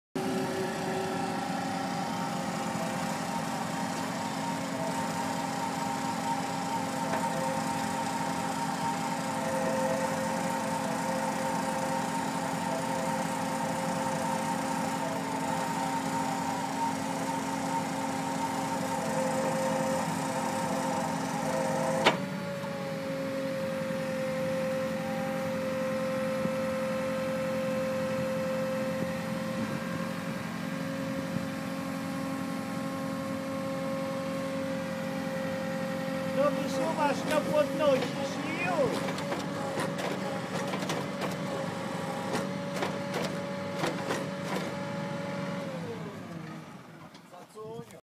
Wiertnica hydrauliczna do koparko ładowarki JCB 3CX
Film prezetuje odwiert wykonany wiertnicą hydrauliczną. Osprzęt dostarcza firma WALZ TRADE